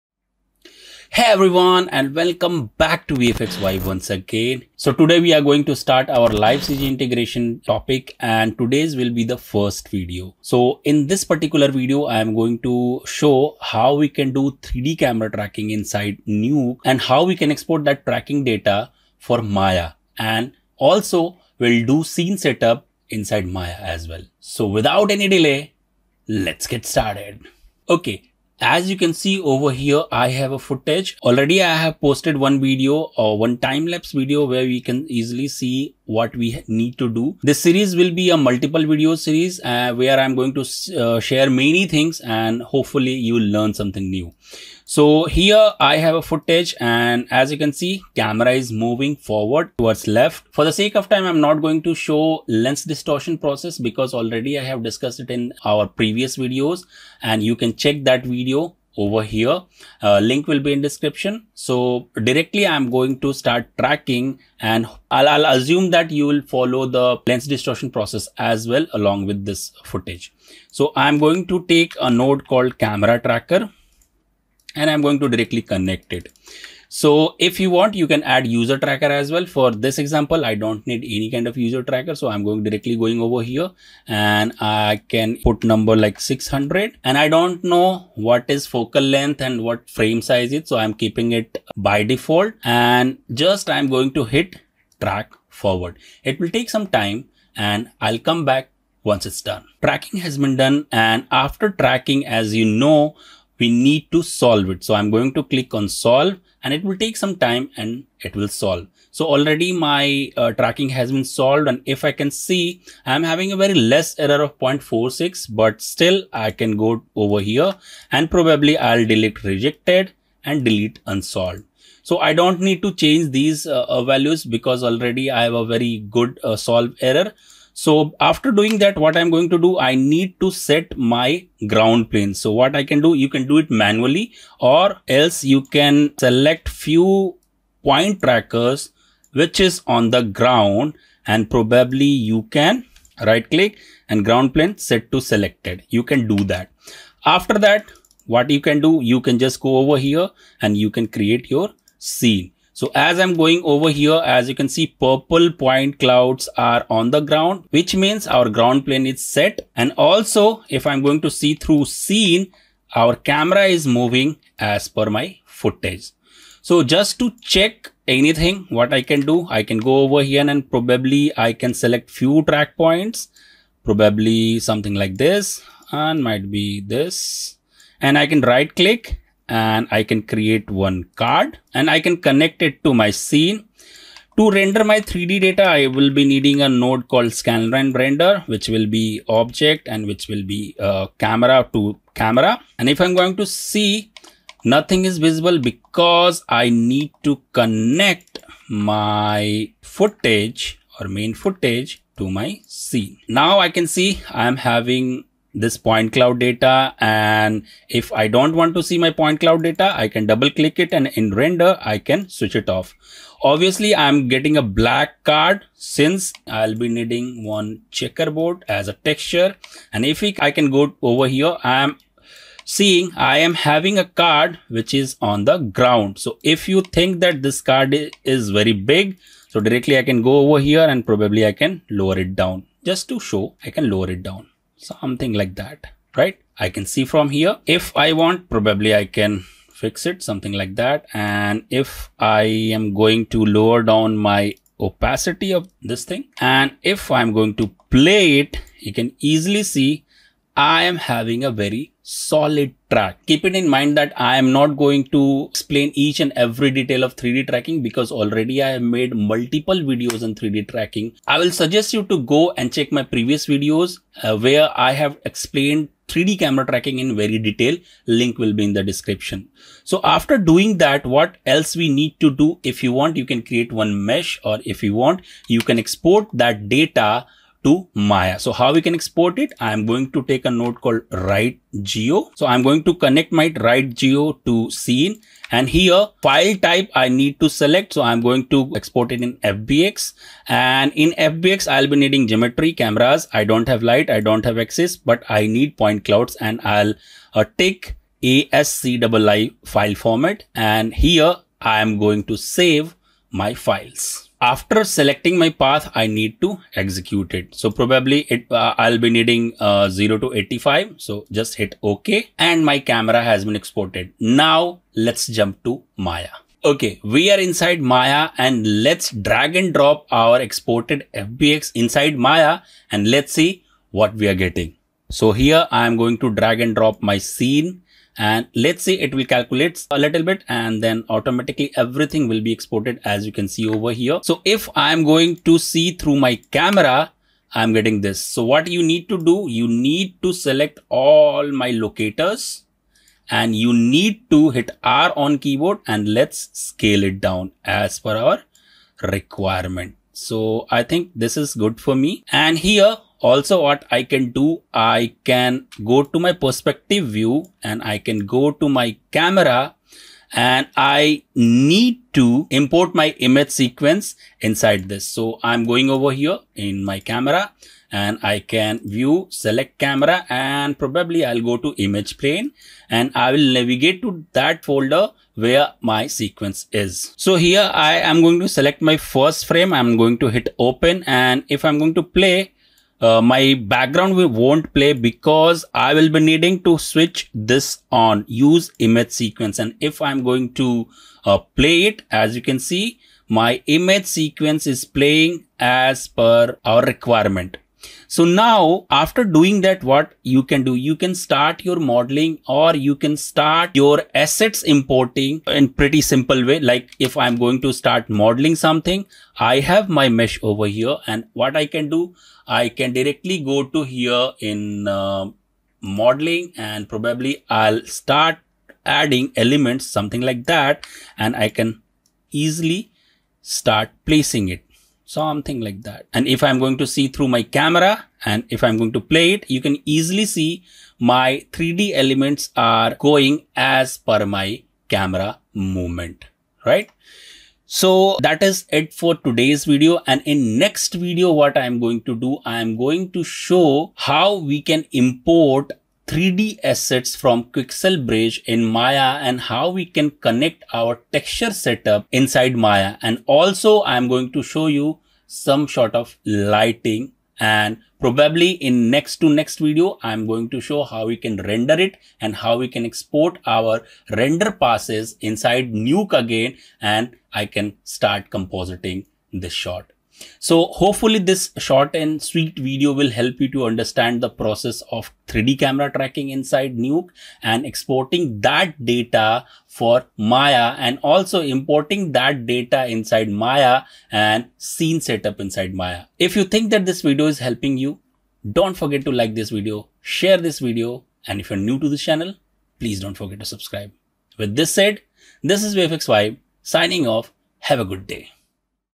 LIVE CG INTEGRATION | PART 1 | 3D CAMERA TRACKING | EXPORTING AND SCENE SETUP IN MAYA | VFX VIBE
Hey Everyone,
as promised, i am launching Live Cg integration Series and in this series we will learn full process.
In part -1 video, i will go through with 3D camera tracking in nuke, exporting tracking data and doing scene setup inside Maya.